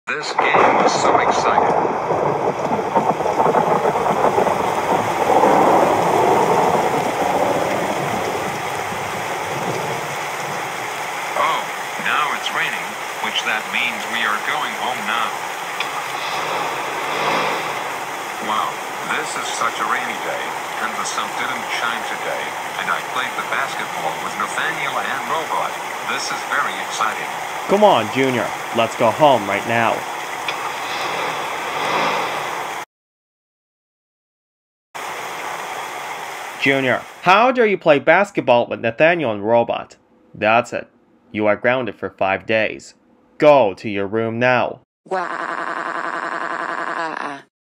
Junior Gets Grounded - Rain Scene
Junior plays Basketball with Nathaniel and Robot and Gets Grounded